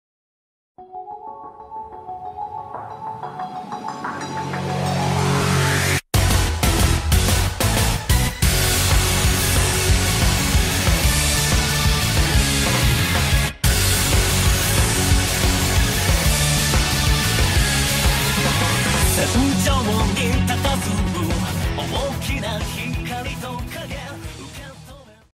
SHF 超人力霸王賽羅 S.H. Figuarts Ultraman Zero｜奧特曼 傑洛 ウルトラマンゼロ｜片頭預告
SHF 超人力霸王賽羅 S.H. Figuarts Ultraman Zero UNBOXING｜奧特曼 傑洛 ウルトラマンゼロ｜開箱分享
有關注我的頻道的人應該都能感受到我非常喜歡ULTRAMAN吧！
先前已經介紹過絕版的一款可動模型

等了這麼久，終於在S.H.Figuarts可動系列盼到了這位最強外掛
円谷的親孫子，子超人力霸王賽文之子！！
「超人力霸王ZERO(ウルトラマンゼロ)」

這次的作品我給予很高的評價
完整影片這幾天會製作上傳
先給大家看片頭吧：）

大家好我是尼克，除了喜歡玩玩具、彈吉他以外
偶爾也會分享、記錄我的日常生活